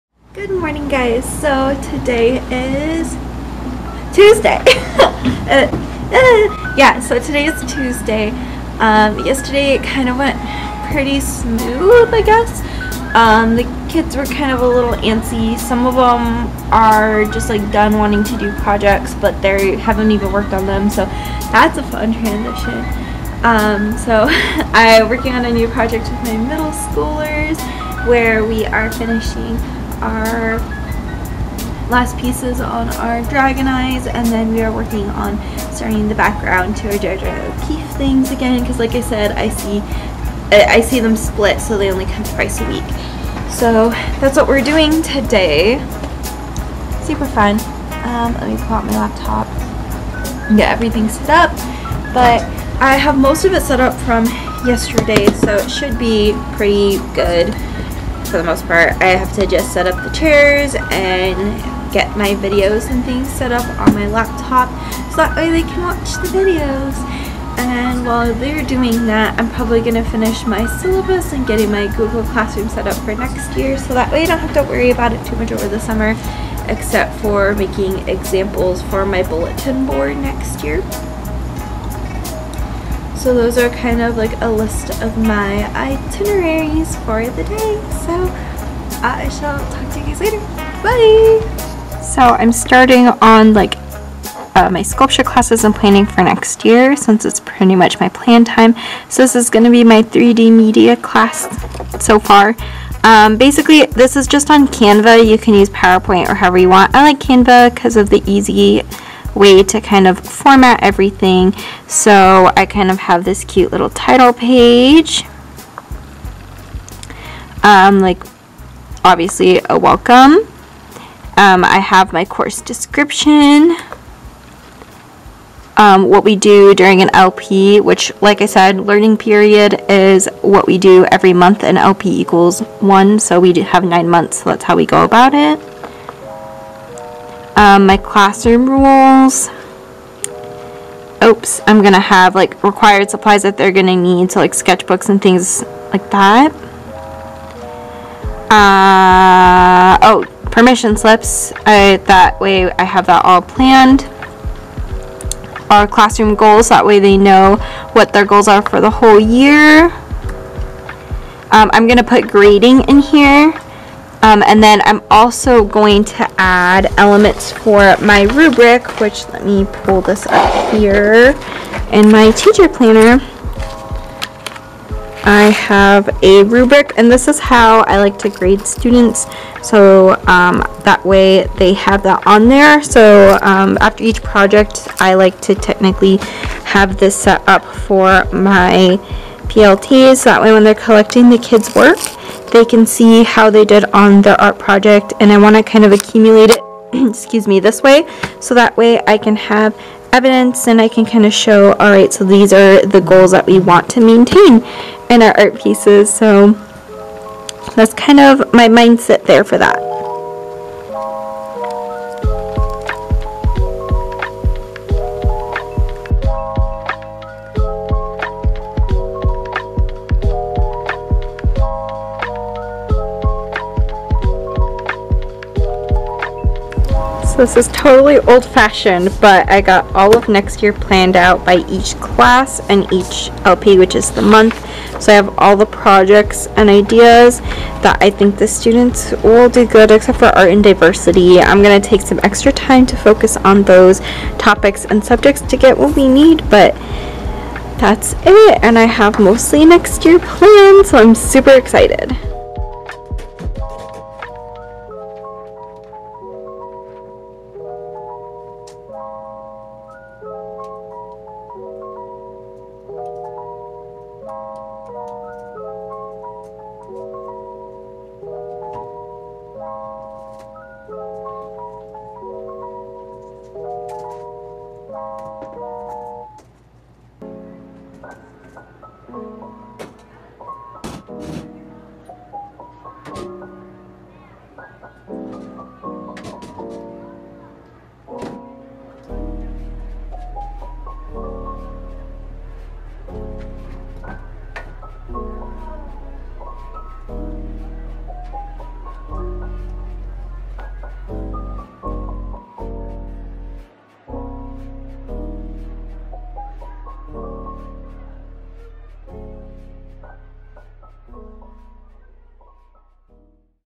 Classroom Vlog Pt. 2 | Tabbytheartsy Bear
In part 2 of this classroom vlog, you can see my daily routines in my art classroom while I show you the projects my students are watching and working on (again!)